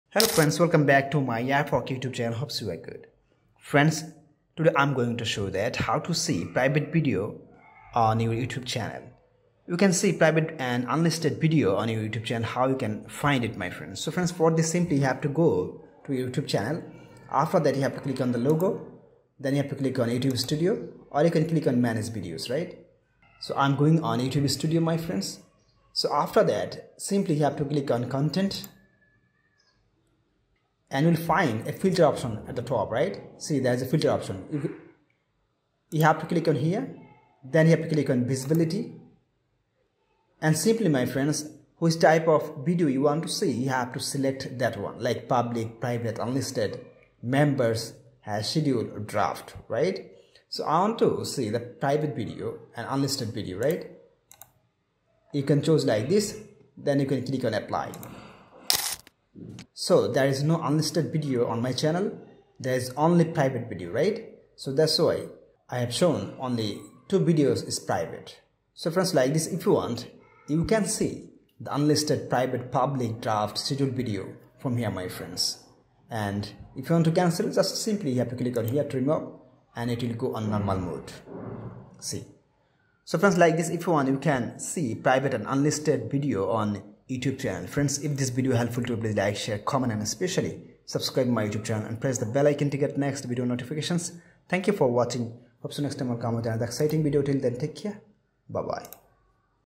How to Watch Private & Unlisted YouTube Videos 🔥 Easy Step by Step Guide 2025
Want to know how to watch private and unlisted YouTube videos that aren’t publicly available? 🤔 In this detailed tutorial, I’ll show you how to access private and unlisted videos on YouTube in 2025 — whether you’re trying to view content shared by a friend or looking for hidden gems!
While private videos are limited to specific users, unlisted videos can be accessed by anyone with the link. So, what’s the difference? And how can you find and view these videos? This guide explains everything you need to know!
🔥 What you'll learn in this video:
• What private vs. unlisted videos are on YouTube
• How to find unlisted videos using a direct link
• How to request access to private videos if you’re invited
• A complete step-by-step guide for both mobile and desktop users
• Why YouTube uses private and unlisted settings for exclusive content
📲 Whether you're on Android, iOS, or using desktop, this tutorial will show you exactly how to access hidden content on YouTube!

From
F HOQUE.